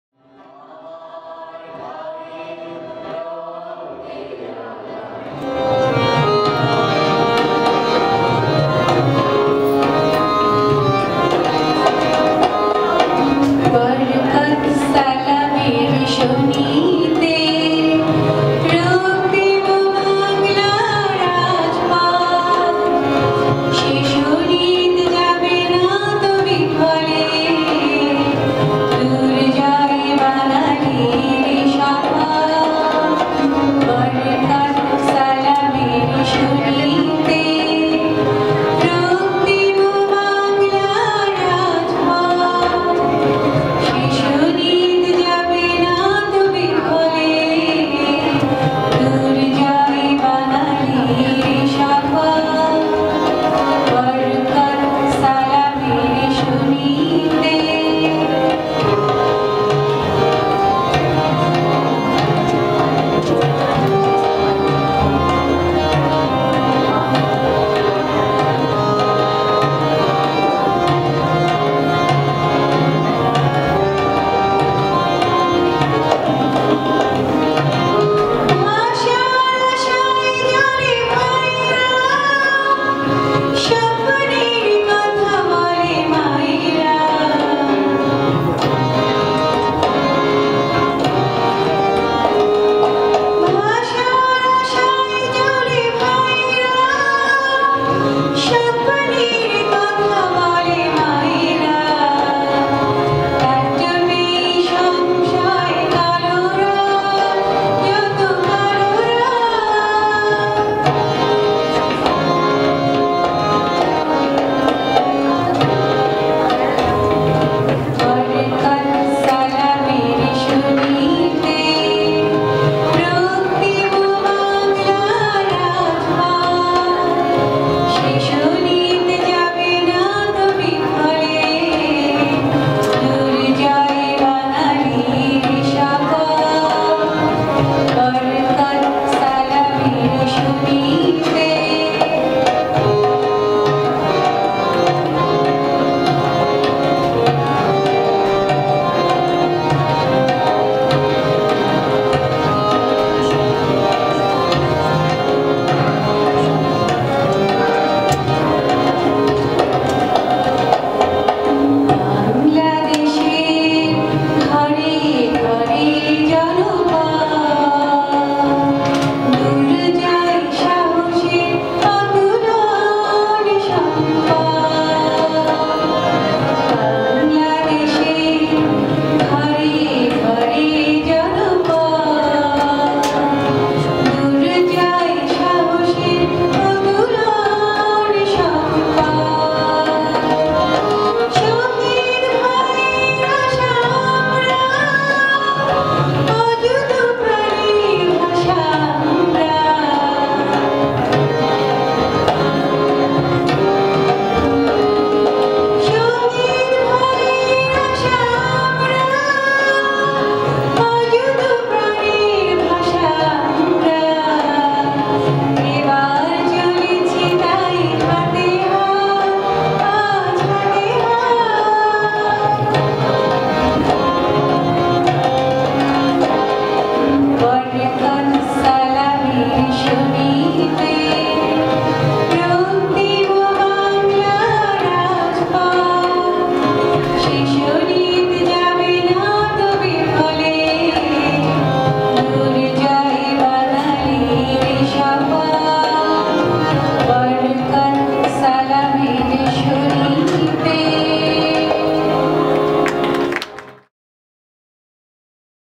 Nargis Akhter Rubi নার্গিস আখতার রুবি
Singer: Nargis Akhter Rubi
একুশে ফেব্রুয়ারী - আন্তর্জাতিক মাতৃভাষা দিবস 2015
Organized by
Bangladesh High Commission, Ottawa
Centre Richelieu-Vanier Community Centre, Ottawa, Canada

Video/Photography + Edited by: Quazi S. Mowla